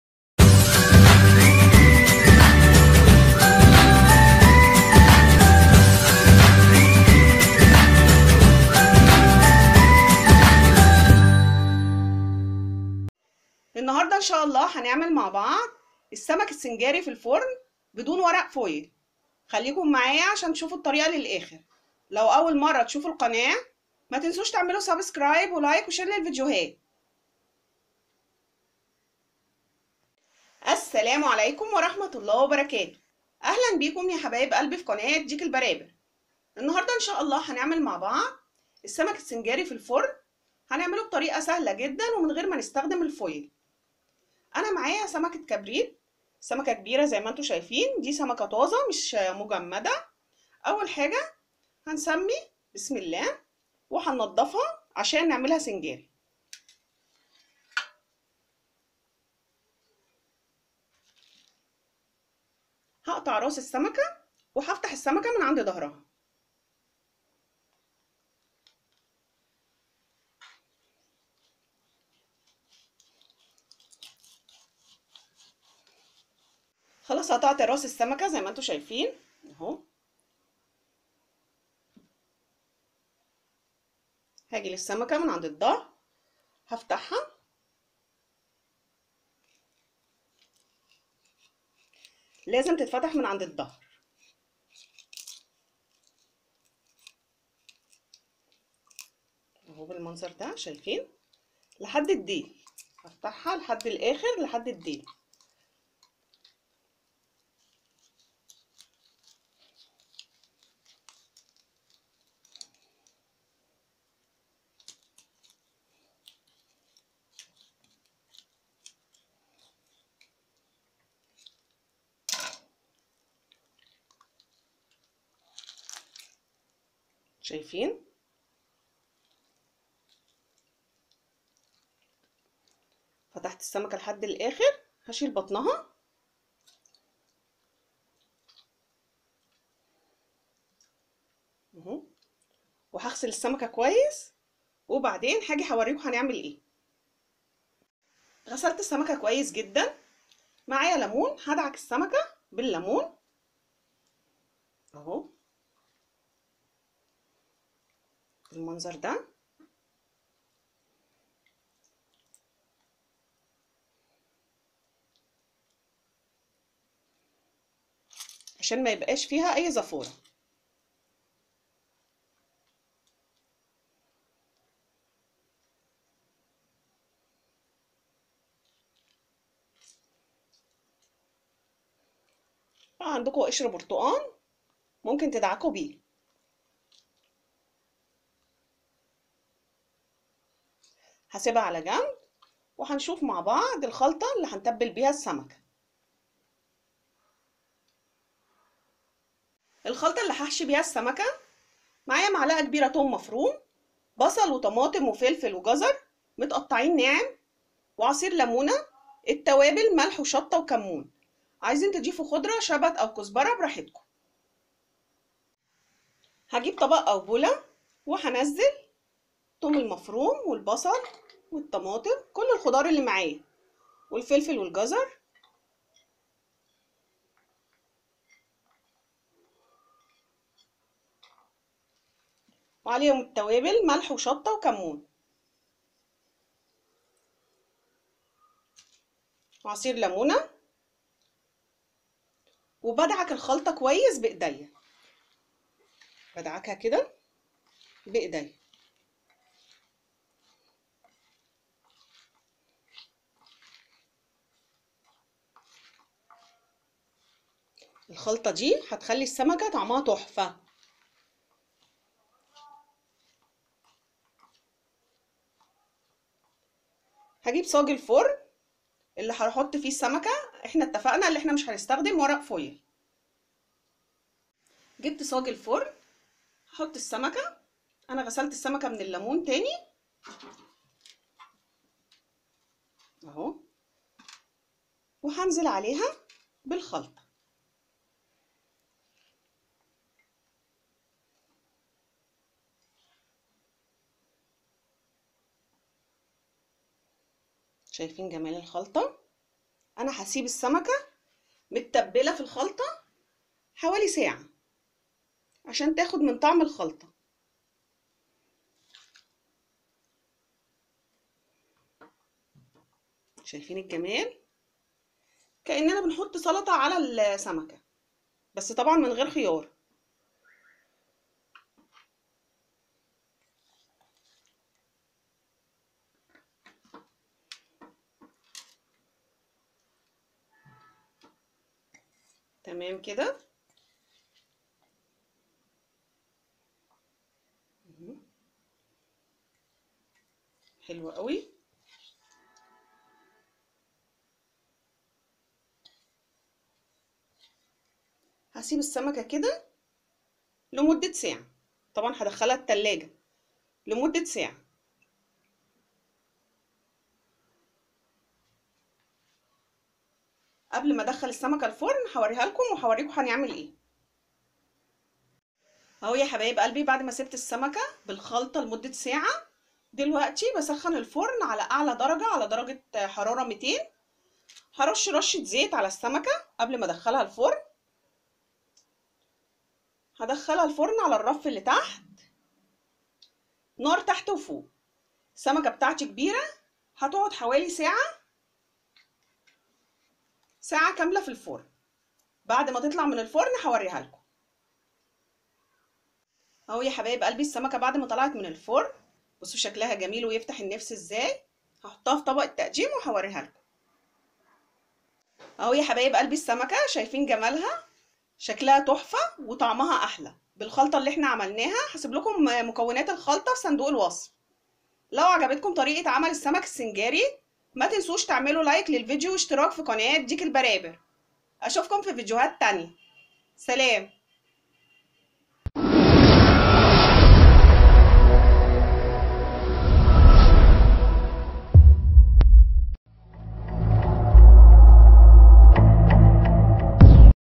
السمك السنجاري في الفرن بدون ورق فويل👌بالخلطة الجباره😋من الألف للياء
خلطة الحشو
بصل مقطع
طماطم مقطعه
فلفل حامي مقطع
جزر مقطع
ملعقة كبيره ثوم مفروم
عصير ليمون
ملح وشطة وكمون